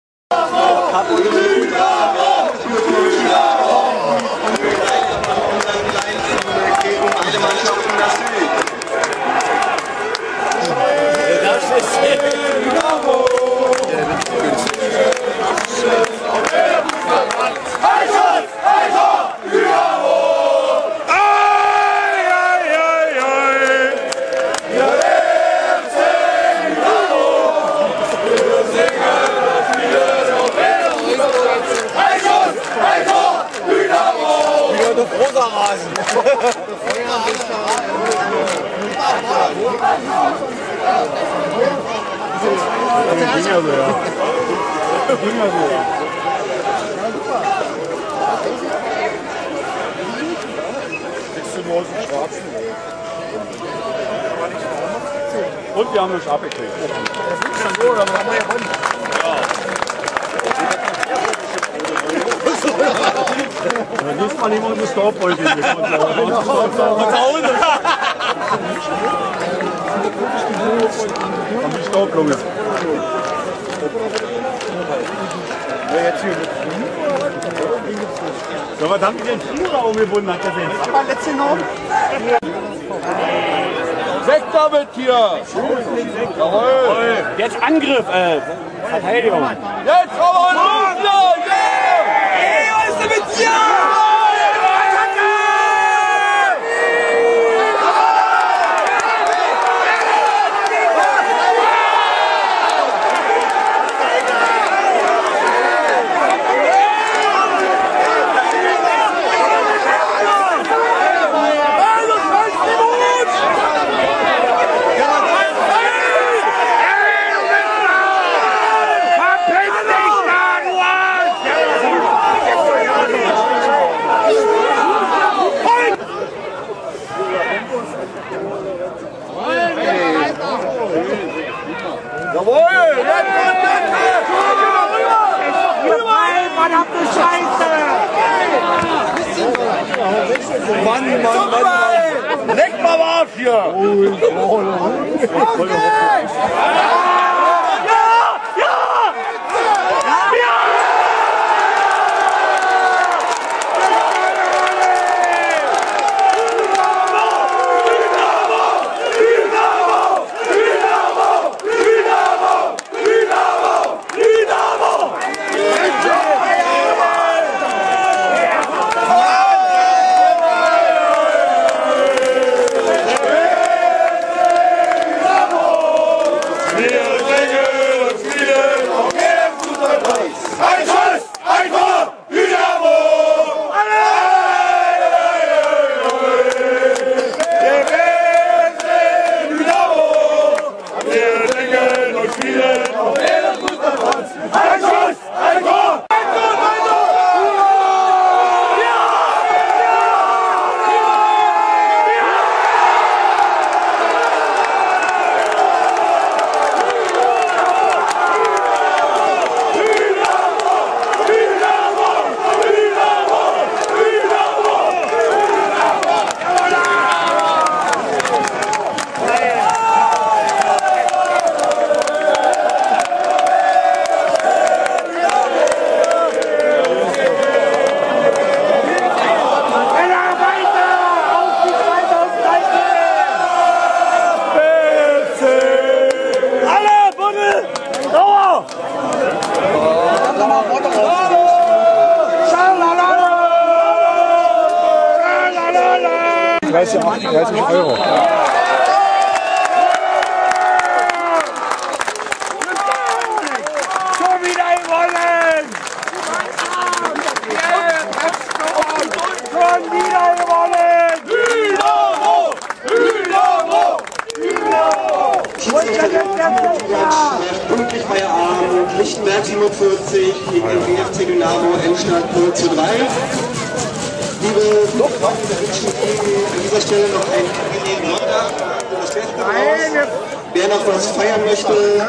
Spielszenen aus dem Derby Lichtenberg 47 gegen BFC Dynamo
Spielszenen vom 23. Spieltag der NOFV-Oberliga Nord am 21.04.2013